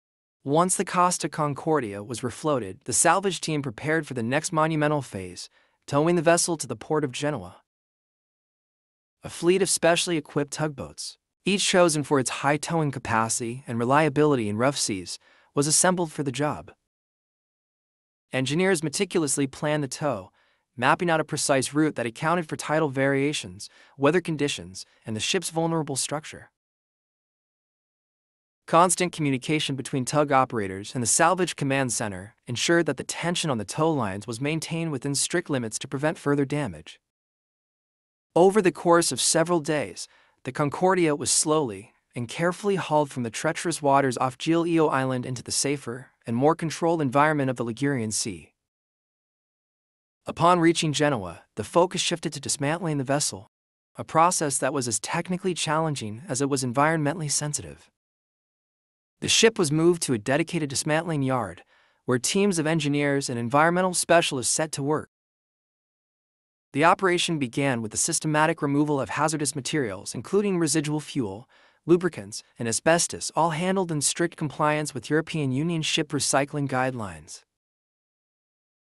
Costa Concordia Salvage: Towing & Transport
We take a look at how engineers towed and transported the Costa Concordia ship to be dismantled and scrapped after the disaster in 2012